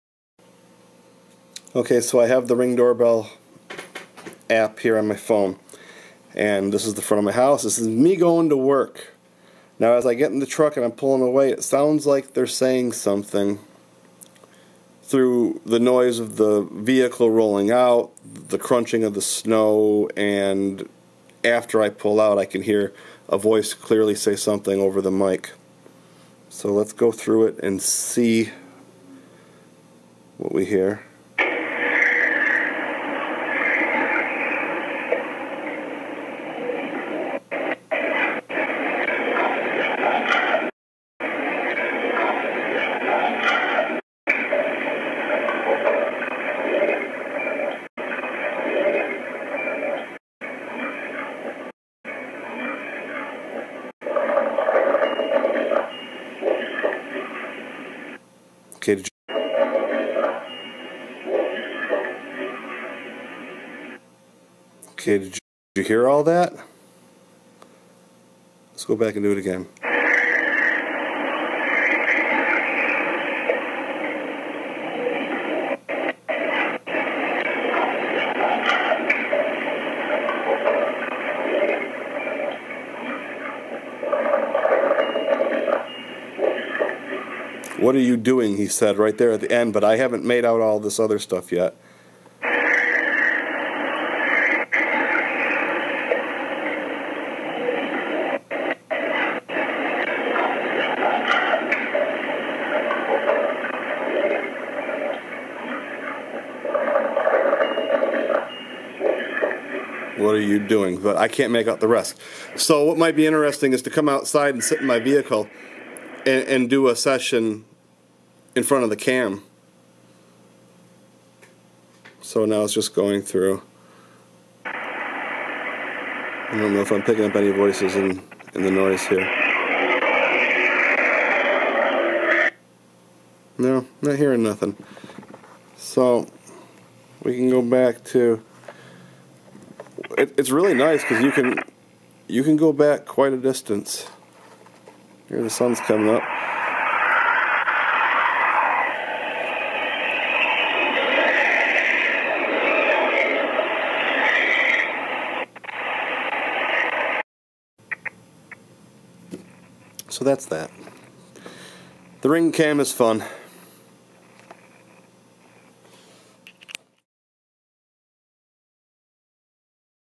Outdoor RIng Cam Spirit voice
A video of my garage door cam capturing spirit voice.